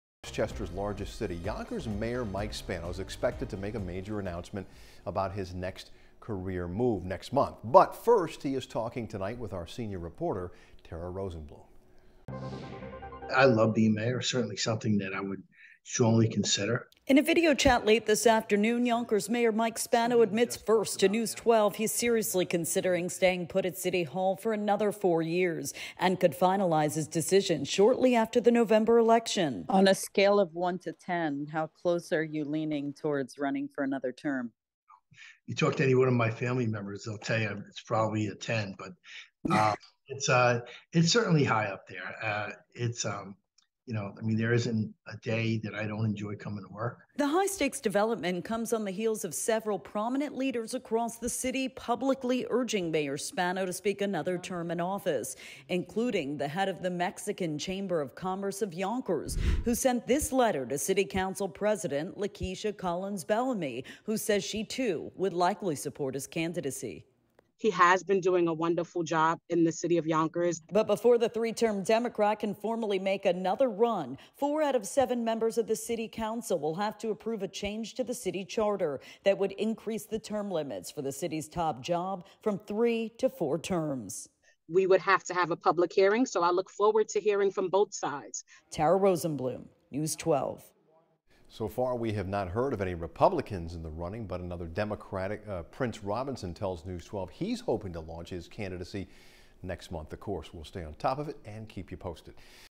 Exclusive: Yonkers Mayor Spano 'strongly' considering running for fourth term
Mayor Mike Spano spoke with to News 12 late Thursday afternoon about his thoughts on staying at City Hall for another four years.